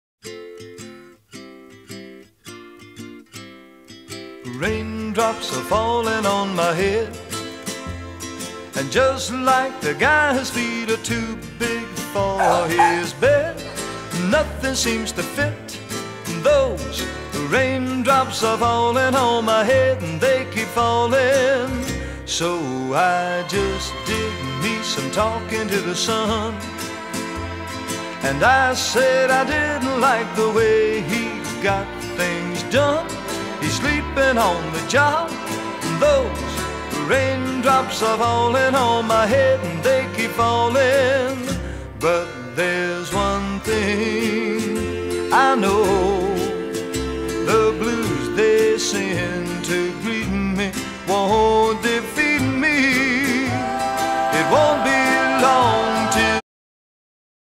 Two Days To Save A Sweet Australian Kelpie Female Shelter Dog!
Candy is running out of time at the shelter and given two days in hopes to find a rescue group to save her. She is not eating in her kennel and needs rescue asap. She is very sweet and beautiful! Candy Thanks You!